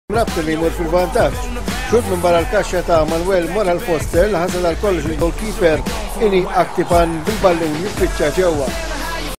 Rabat Ajax vs Birkirkara F.C. - Highlight 1 (2012/13 Round 2)
Data: Is-Sibt, 8 ta' Diċembru, 2012
Logħba: Rabat A. vs Birkirkara FC
Riżultat: 3-3
Formazzjoni: Clifford Gauci (85' Chris Vella), David Azzopardi, Dylan Falzon, Ivan Carapic, Joseph Caruana, Justin Felice, Luis Armario Negrin, Manuel Moral Fuster, Patrick Borg (88' Gaetano Gesualdi), Sean Mintoff, Yan Cauchi (71' Wayne Borg)
Skurjaw: Manuel Moral Fuster (3'), Justin Felice (75'), Luis Armario Negrin (87')

Manuel Moral Fuster passes the ball to Luis Armario Negrin who returns the ball back.  Fuster shoots for a splendid first goal of the match.